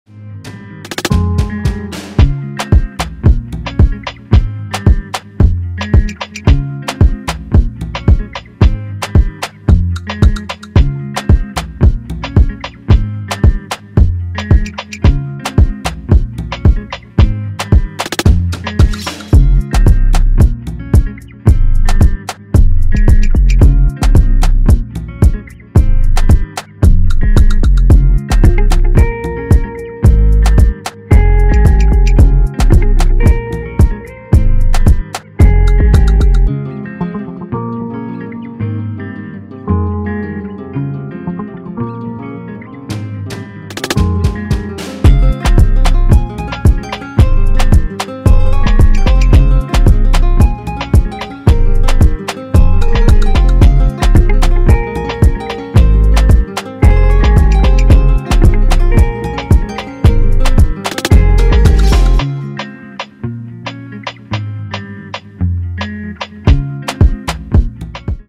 Men's Winter Bomber Snow Suit Prime Baggy Snowboard Jacket & Pants
The bomber jacket boasts superior insulation and weather-resistant materials to keep you warm and dry. Its baggy fit ensures unrestricted movement, while the multiple cargo pockets offer ample storage for all your essentials. Paired with matching baggy snowboard pants, this set combines style, comfort, and practicality, making it perfect for hitting the slopes in style.